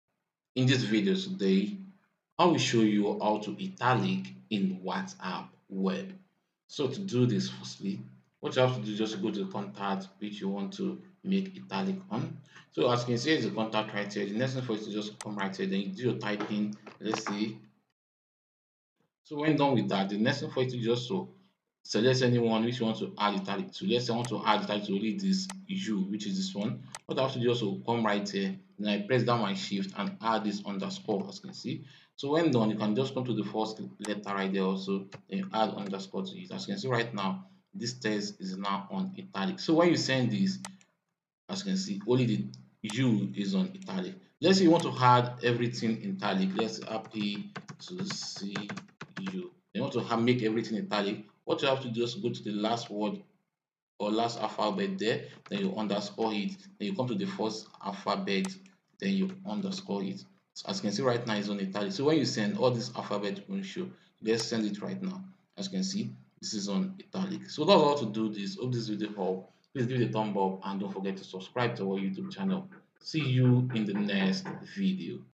How To Italic In WhatsApp Web
I will show you how to italic in WhatsApp web.